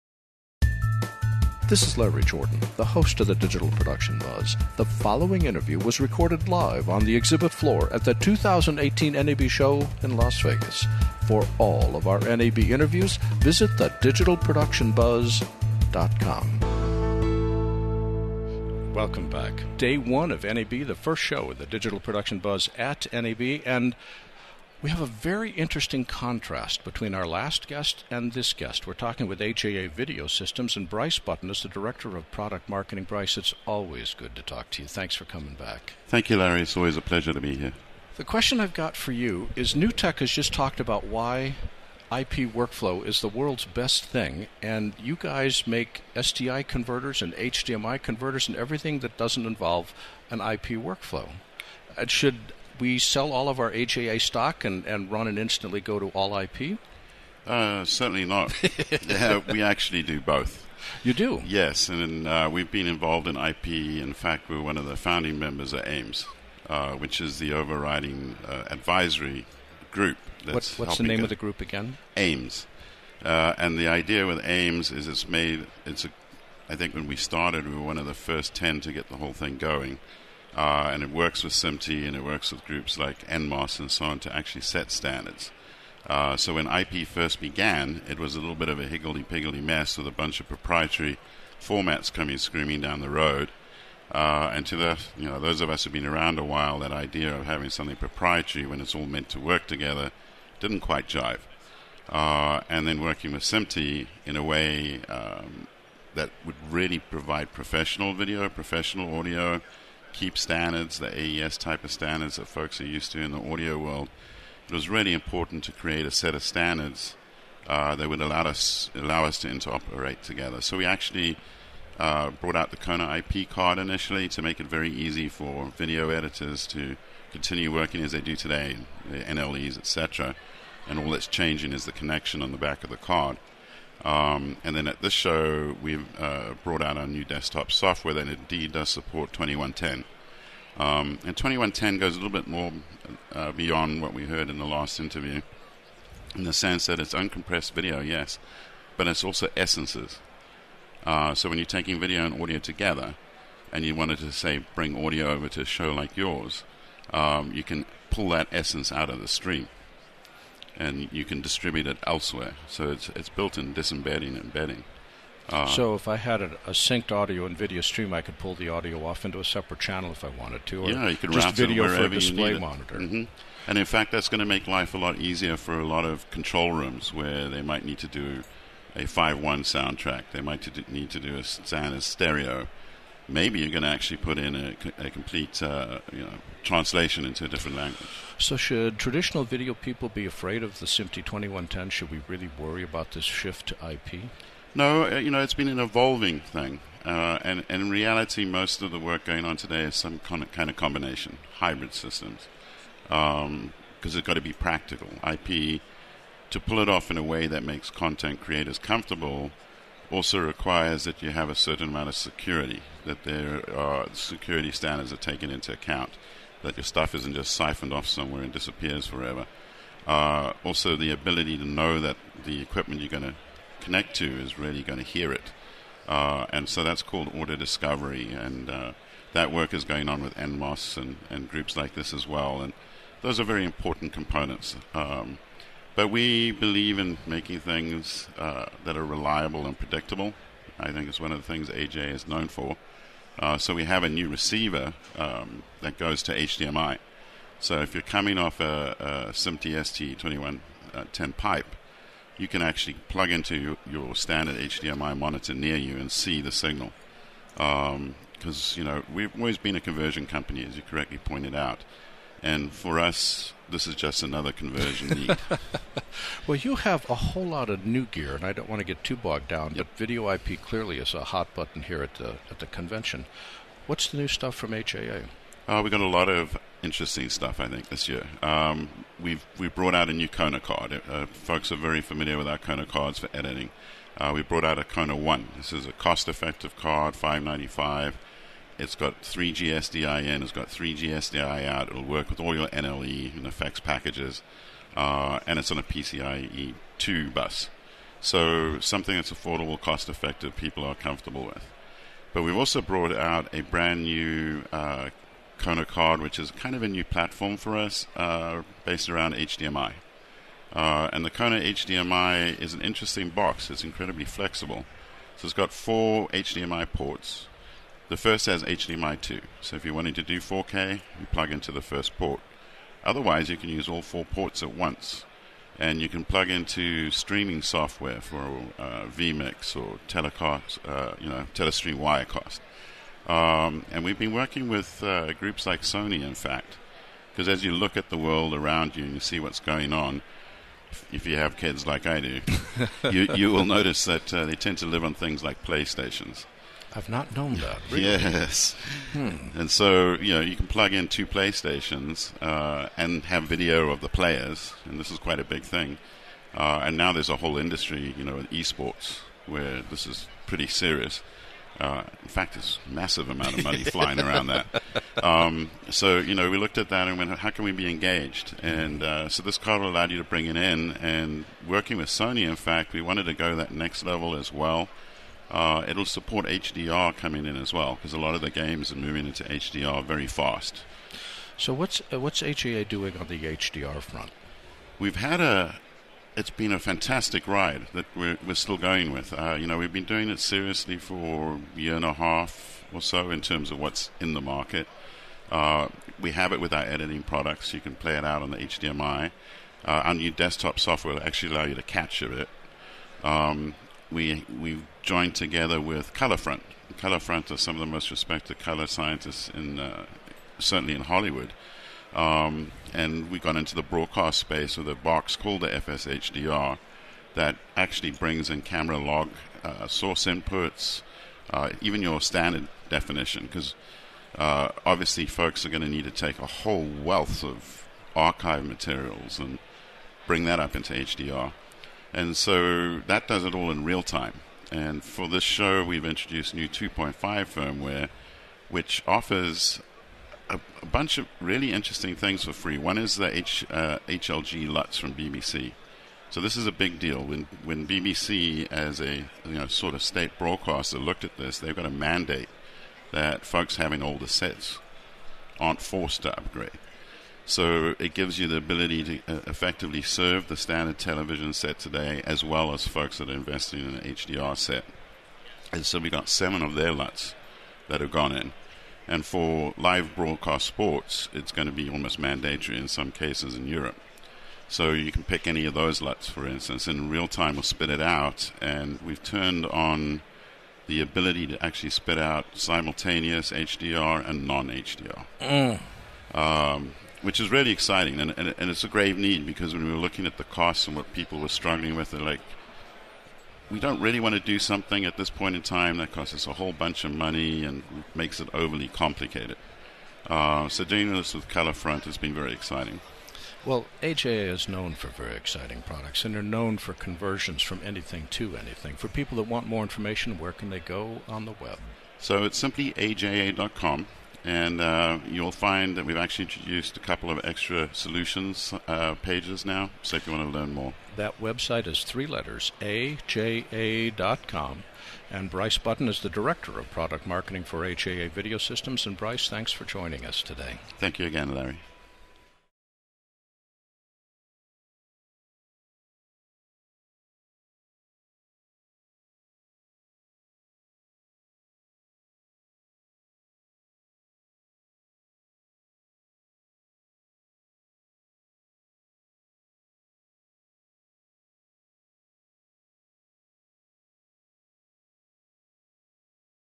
Bryce Button, NAB 2018 Audio Interview
Bryce Button talks about the shift from video to IP, AJA’s new KONA card and the balance between a traditional video workflow and the new video over IP.